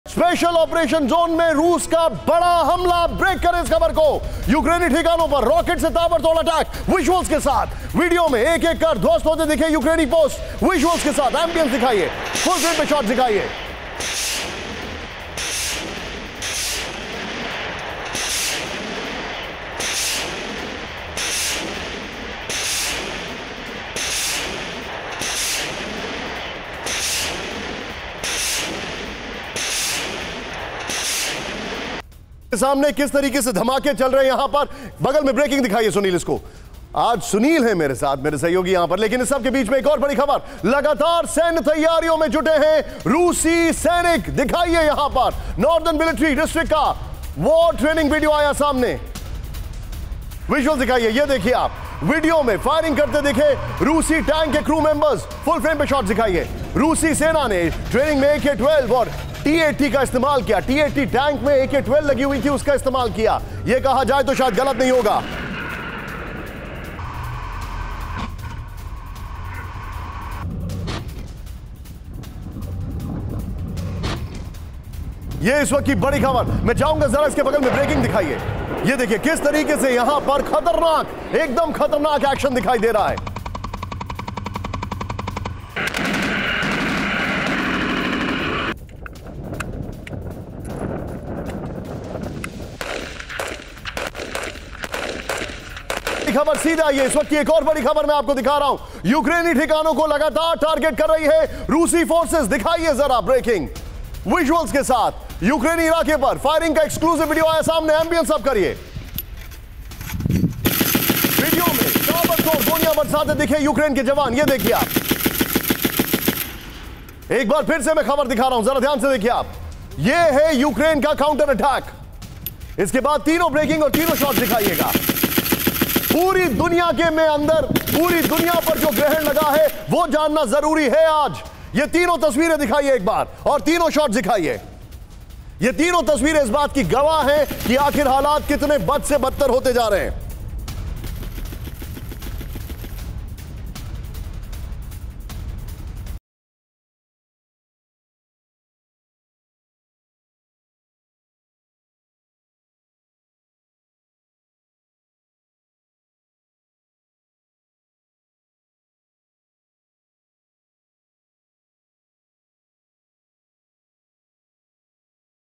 स्पेशल ऑपरेशन जोन में Russia का बड़ा हमला, यूक्रेनी ठिकानों पर Rocket से ताबड़तोड़ अटैक
स्पेशल ऑपरेशन जोन में रूस का बड़ा हमला
यूक्रेनी ठिकानों पर रॉकेट से ताबड़तोड़ अटैक
वीडियो में एक एक कर ध्वस्त होते दिखे यूक्रेनी पोस्ट